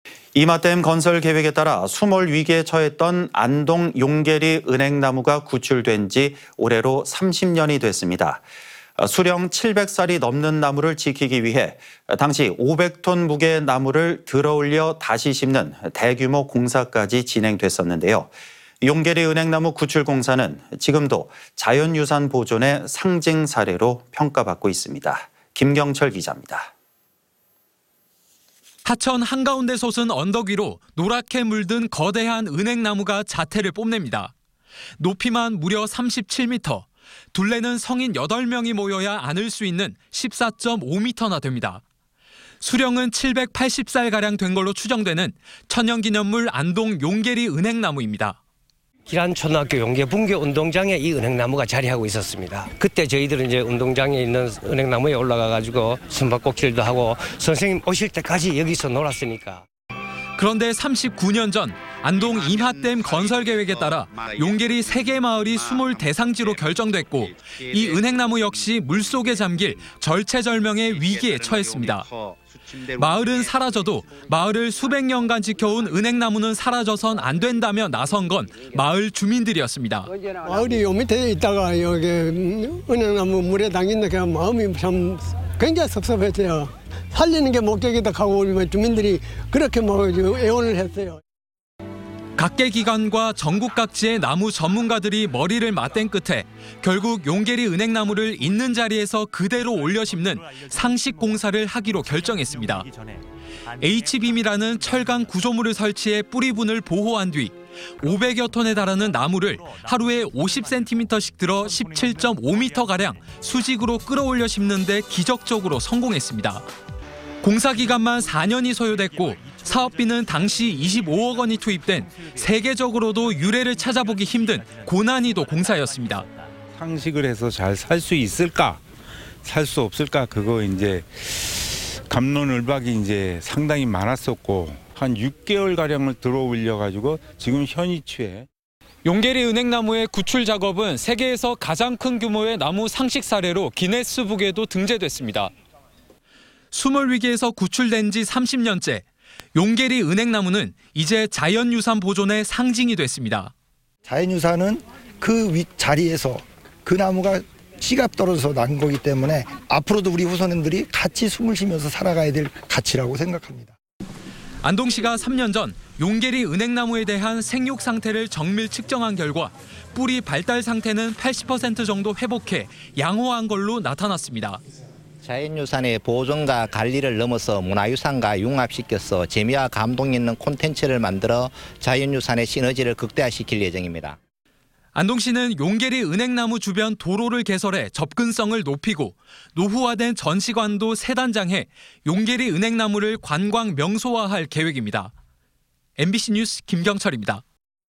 [R]용계리 은행나무 구출 30년.."자연유산 보존 상징" / 안동MBC
2024/11/05 08:30:00    작성자 : 김경철

◀ 앵 커 ▶

임하댐 건설 계획에 따라 수몰 위기에 처했던 안동 용계리 은행나무가 구출된 지
올해로 30년이 됐습니다.

수령 7백 살이 넘는 나무를 지키기 위해
당시 5백 톤 무게의 나무를 들어올려 다시 심는 대규모 공사까지 진행됐었는데요.

용계리 은행나무 구출 공사는 지금도 자연유산 보존의 상징 사례로 평가받고 있습니다.

김경철 기자

◀ 리포트 ▶

하천 한가운데 솟은 언덕 위로,
노랗게 물든 거대한 은행나무가
자태를 뽐냅니다.

높이만 무려 37m, 둘레는 성인 8명이 모여야
안을 수 있는 14.5m나 됩니다.

수령은 780살가량 된 걸로 추정되는
천연기념물, 안동 용계리 은행나무입니다.

◀ INT ▶ 권광혁 / 안동 용계리 주민
"길안초등학교 용계분교 운동장에 이 은행나무가 자리하고 있었습니다. 그때 저희들은 운동장에 있는 은행나무에 올라가서 숨바꼭질도 하고, 선생님 오실 때까지 여기서 놀았으니까..."

그런데 39년 전, 안동 임하댐 건설 계획에 따라 용계리 3개 마을이 수몰 대상지로 결정됐고,
이 은행나무 역시 물속에 잠길 절체절명의
위기에 처했습니다.

마을은 사라져도, 마을을 수백 년간 지켜 온
은행나무는 사라져선 안 된다며 나선 건
마을 주민들이었습니다.

◀ INT ▶ 권오설 / 안동 용계리 주민
"마을이 이 밑에 있다가 은행나무 물에 잠긴다고 해서 마음이 참 굉장히 섭섭했어요. '(은행나무를) 살리는 게 목적이다' 라고 주민들이 그렇게 애원을 했어요."

각계 기관과 전국 각지의 나무 전문가들이
머리를 맞댄 끝에, 결국 용계리 은행나무를
있는 자리에서 그대로 올려 심는 '상식' 공사를
하기로 결정했습니다.

H빔이라는 철강 구조물을 설치해
뿌리 분을 보호한 뒤,
500여 톤에 달하는 나무를 하루에 50cm씩
들어 17.5m가량 수직으로 끌어올려
심는 데 기적적으로 성공했습니다.

공사 기간만 4년이 소요됐고,
사업비는 당시 25억 원이 투입된 세계적으로도 유례를 찾아보기 힘든 고난이도 공사였습니다.

◀ INT ▶ 이동석 / 당시 상식 담당 업체
"상식을 해서 잘 살 수 있을까, 살 수 없을까 갑론을박이 상당히 많았었고, 한 6개월 가량을 들어올려서 지금 현 위치에..."

[S/U]
"용계리 은행나무의 구출 작업은
세계에서 가장 큰 규모의 나무 상식 사례로
기네스북에도 등재됐습니다."

수몰 위기에서 구출된 지 30년 째,
용계리 은행나무는 이제 자연유산 보존의
상징이 됐습니다.

◀ INT ▶ 조운연 / 국가유산청 자연유산위원회 위원
"자연유산은 그 자리에서, 그 나무의 씨가 떨어져서 난 것이기 때문에 앞으로도 우리 후손들이 같이 숨을 쉬면서 살아가야 할 가치라고 생각합니다."

안동시가 3년 전, 용계리 은행나무에 대한
생육상태를 정밀 측정한 결과, 뿌리 발달상태는 80% 정도 회복해 양호한 걸로 나타났습니다.

◀ INT ▶ 권기창 / 안동시장
"자연유산의 보존과 관리를 넘어 문화유산과
융합시켜서 재미와 감동 있는 콘텐츠를 만들어 자연유산의 시너지를 극대화할 예정입니다."

안동시는 용계리 은행나무 주변 도로를 개설해 접근성을 높이고, 노후화된 전시관도 새단장해
용계리 은행나무를 관광 명소화할 계획입니다.

MBC뉴스 김경철입니다. (영상취재 차영우)